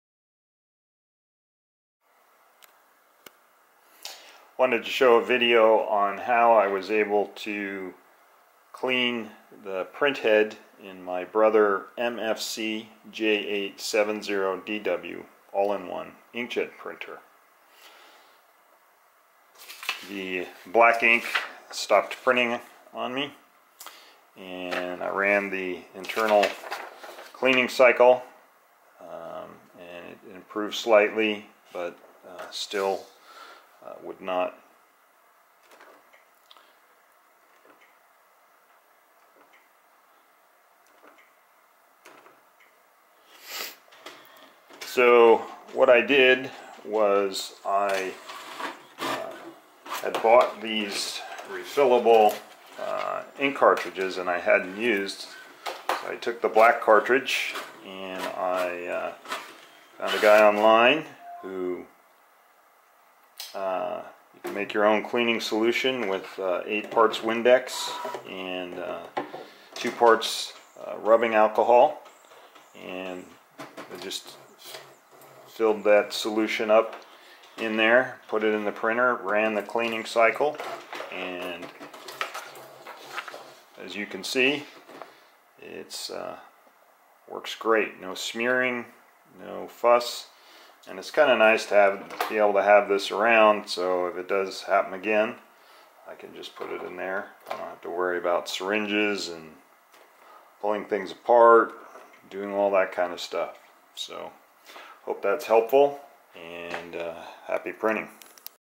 Brother MFC-J870DW A Better Way to Clean the Printhead
DIY - How to unclog Brother MFC J870DW All in One Inkjet Print head. A better way to clean, unclog and maintain print head using a refillable cartridge
If you are ready to throw away your printer here is a way to unclog the print head
Thanks to Rob's Home Life for DYI print head cleaning solution

If you own a Inkjet Printer it is not a matter of if but when the print head will clog. I found a better way to clean them without getting ink allover your hands.
MFC J870DW Print Head Cleaning and unclog